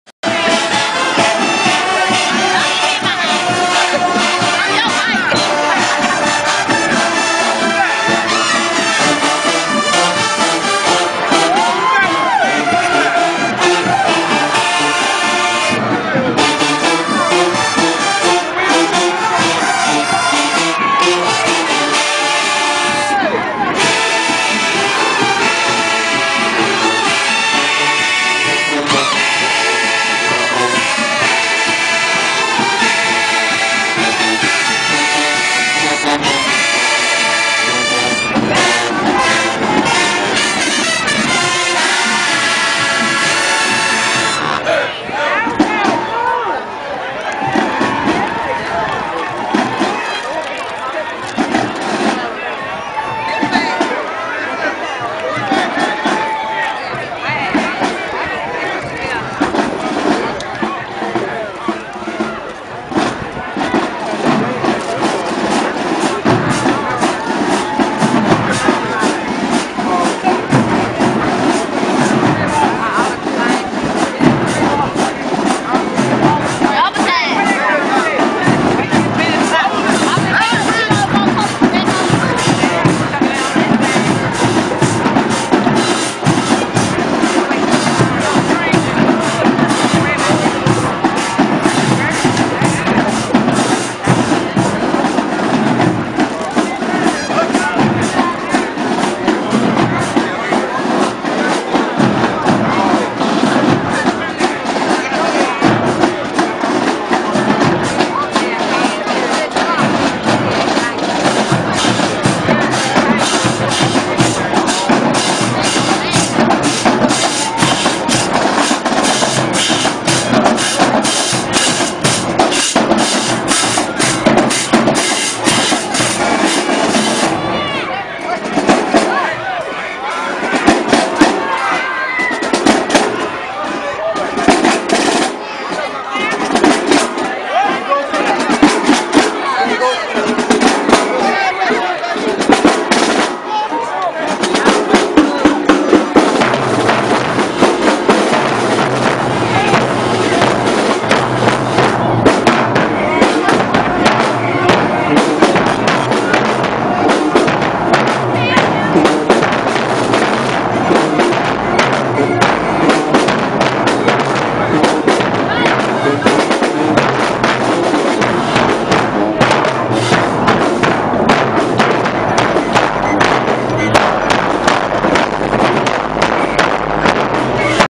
Southern Heritage Classic 080 Talladega College Marching Tornado Band
The Talladega College Marching Band is actually quite good, and has been a frequent visitor to Memphis over the last several years, appearing in the Southern Heritage Classic Parade, local band battles, and the Whitehaven Christmas Parade last year.